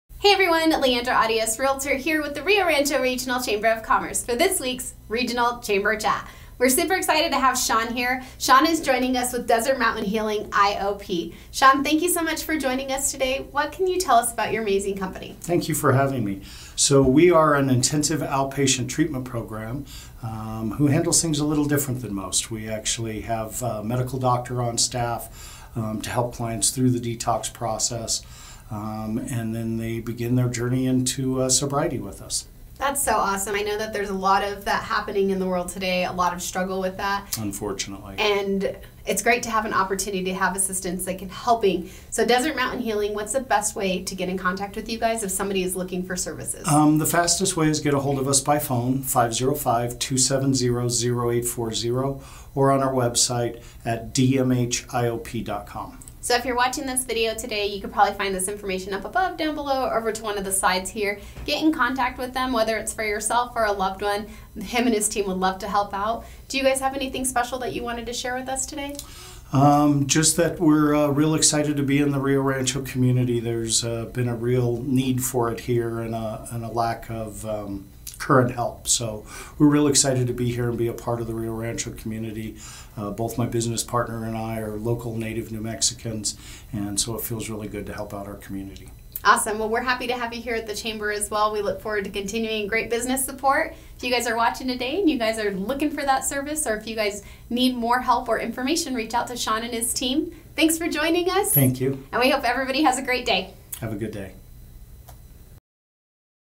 Chamber Chat - Desert Mountain Healing IOP
Chamber Chat with your local guide LeAndra Arias, Realtor and Sean Roberts, Owner of Desert Mountain Healing IOP. Make sure to like and follow this great local business!

Contact LeAndra for all your real estate needs!